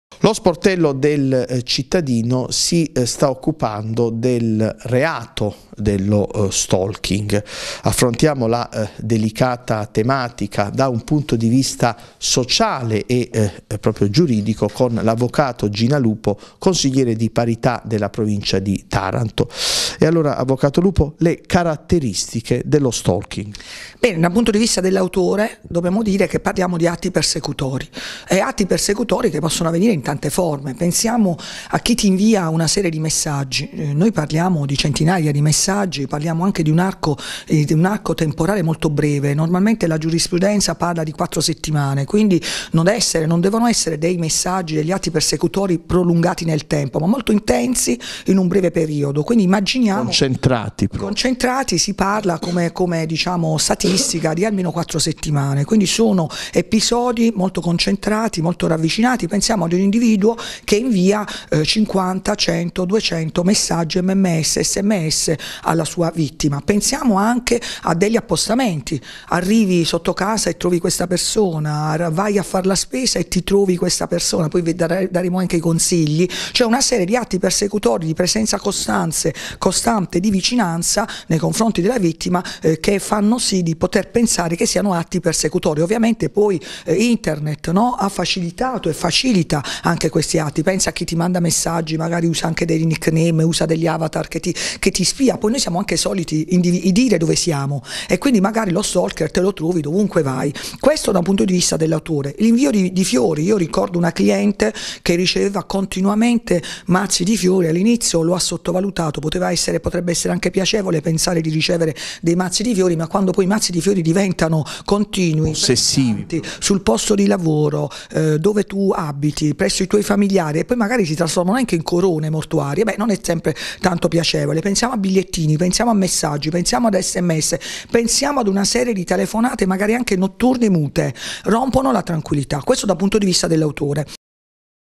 Stalking: Le sue caratteristiche e la figura dello Stalker
Stalking: Le sue caratteristiche e la figura dello Stalker - Avv. Gina Lupo - Consigliera di Parità Provincia di Taranto